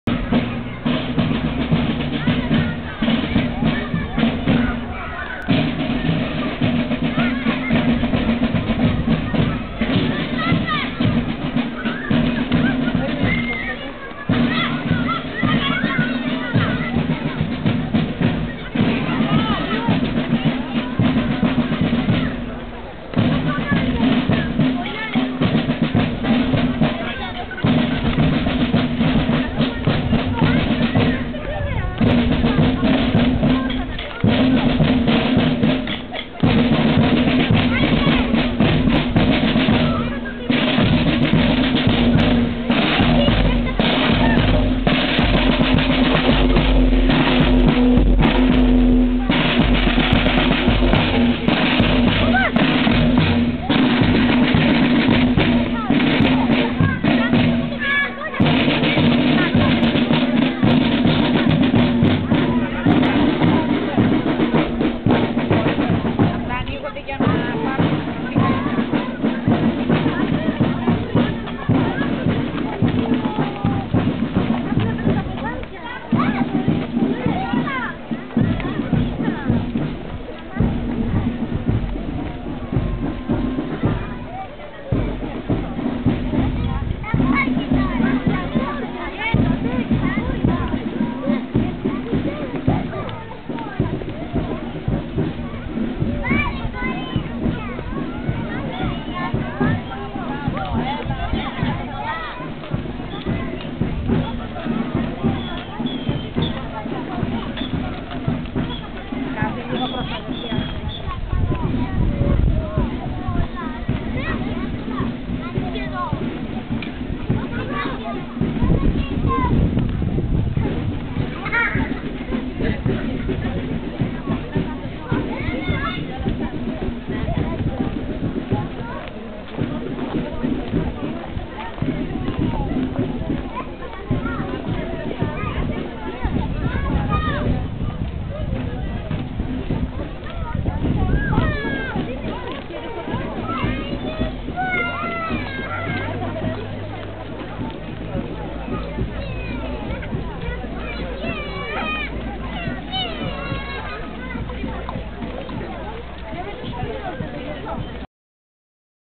28η Οκτωβριου στην Πυλο
28η Οκτωβριου  2015 στην Πυλο  ,στον εορτασμο της Εθνικής εορτής  28η Οκτωβριου 1940